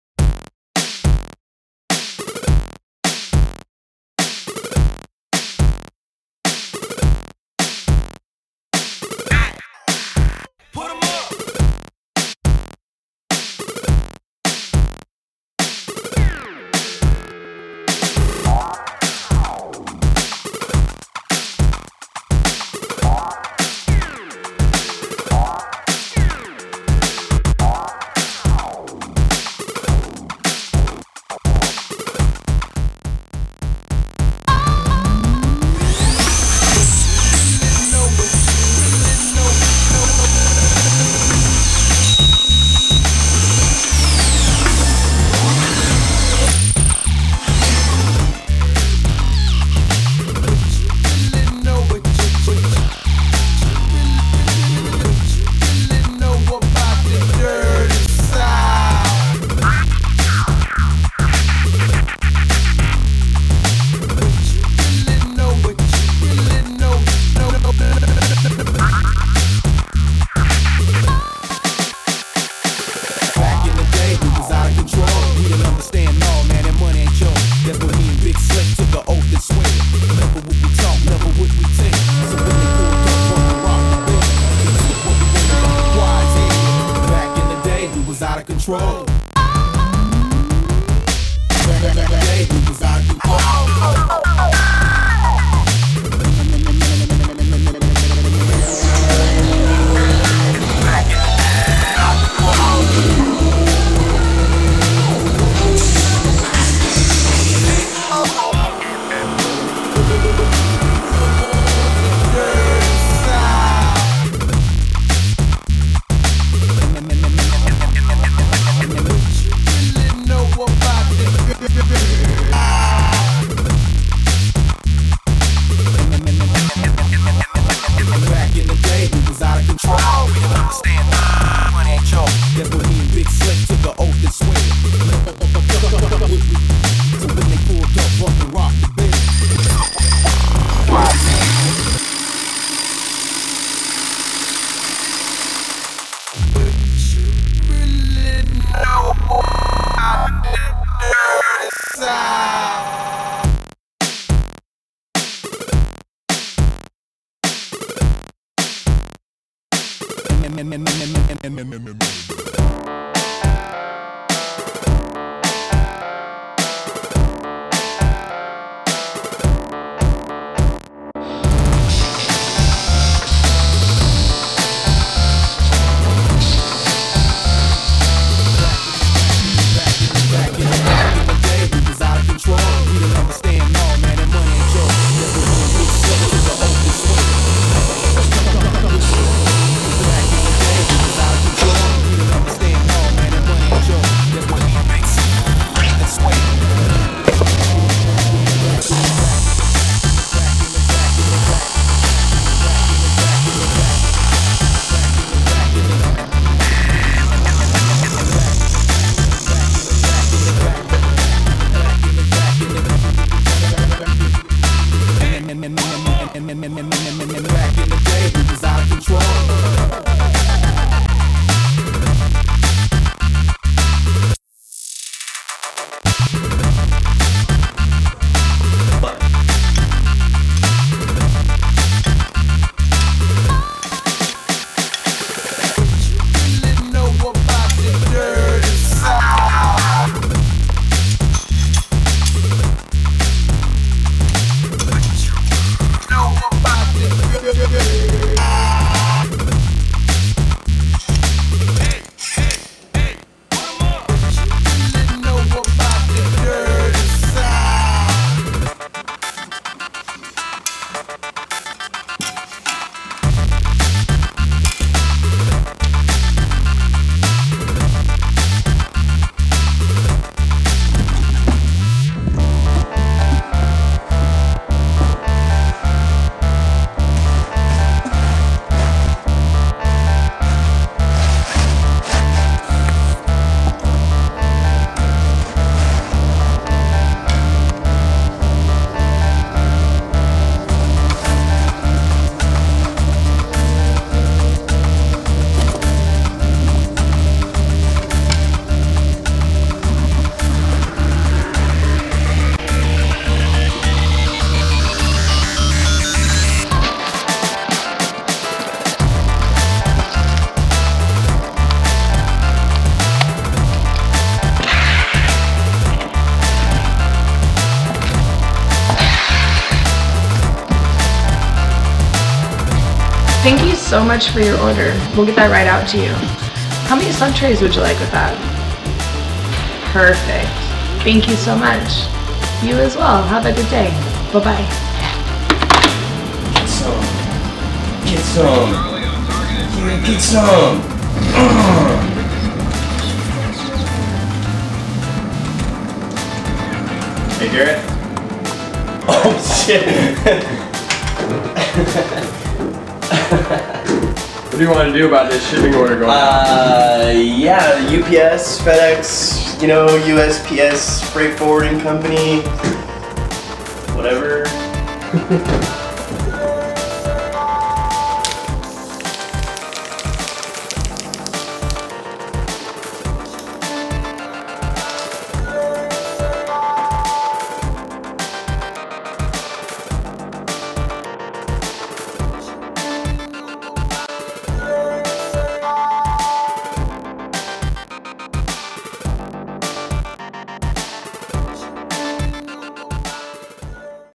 The Crane Stand : A Day In The Life
An inside look at how the best laptop stand on the market is brought to life.

Video featured on Scratchworx